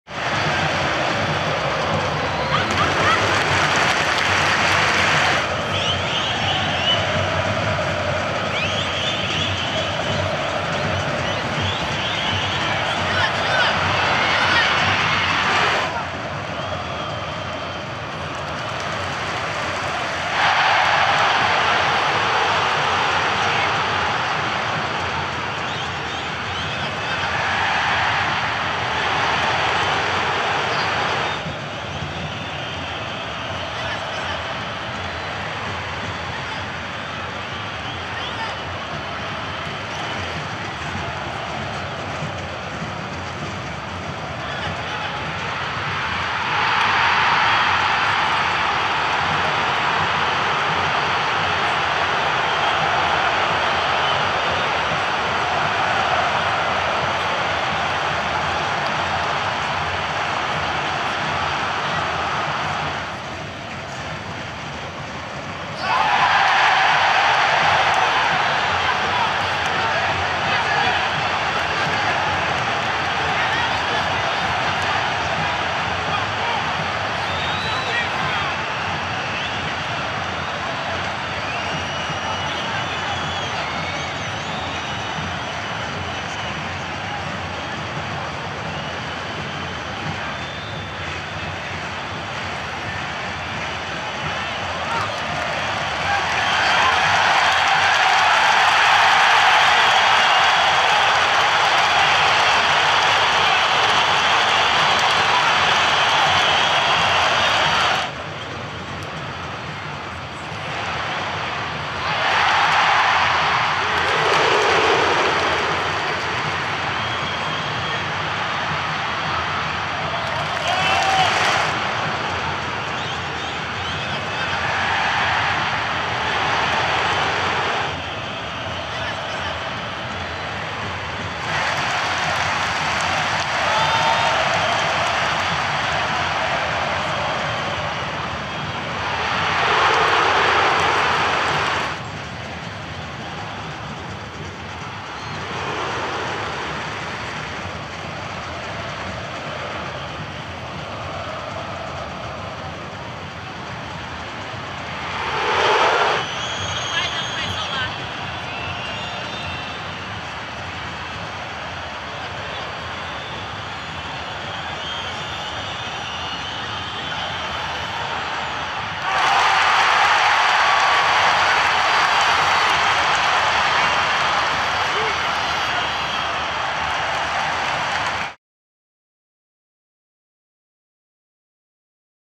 GALO FAZ GOL BEM ANULADO E TEM JOGADOR EXPULSO AINDA NO 1º TEMPO | CORINTHIANS 0X0 ATLÉTICO-MG
Com um homem a mais desde o fim do primeiro tempo, o Corinthians não conseguiu fazer gol da vitória na primeira rodada do Brasileirão

Crédito: Eduardo Carmim/Photo Premium/Gazeta Press